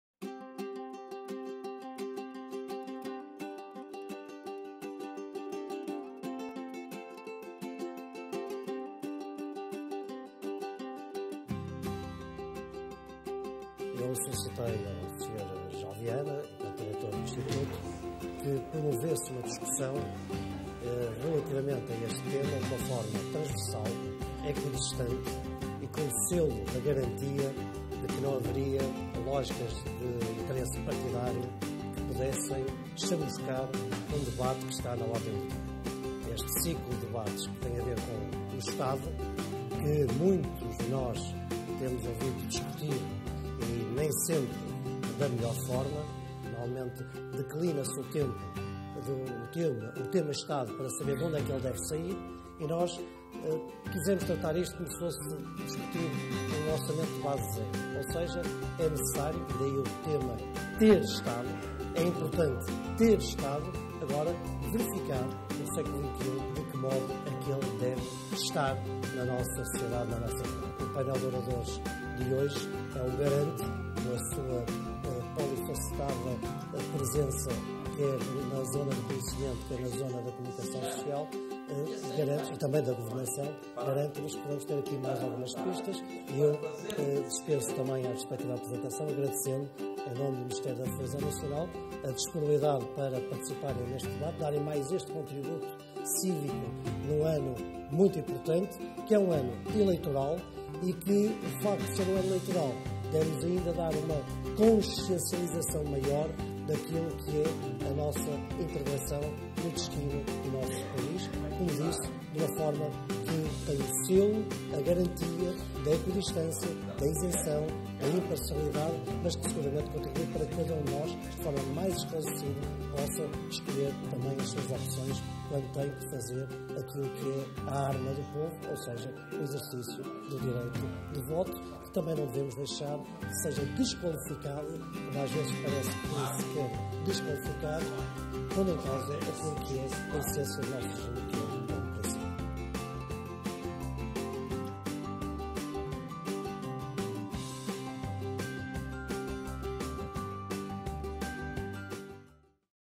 "TER ESTADO" - Debate sobre Política Externa, Defesa e Recursos Estratégicos Nacionais
Decorreu, esta manhã, o sexto debate público TER ESTADO, sob o tema: “Política Externa, Defesa e Recursos Estratégicos Nacionais: Como exercer soberania hoje?”

A sessão teve, como conferencistas convidados, o general Loureiro dos Santos, o ex-ministro da Defesa Nacional, Luís Amado, o jornalista, Nuno Rogeiro e o professor Vasco Rato.